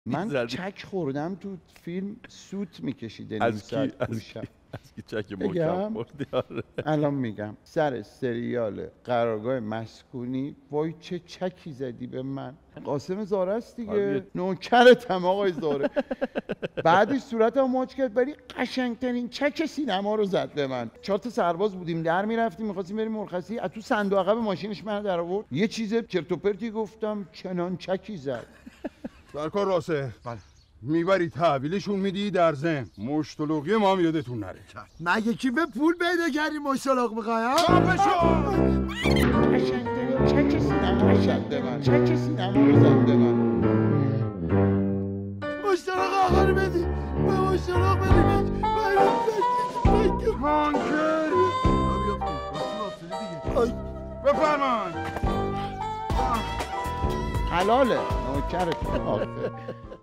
سیلی محکم قاسم زارع تو گوش مهران غفوریان
مهران غفوریان و چک افسری ای که به بدترین شکل ممکن تو صورتش کوبیده شد
مهران غفوریان اذعان کرد که تا مدت‌ها حالش بد بوده 😂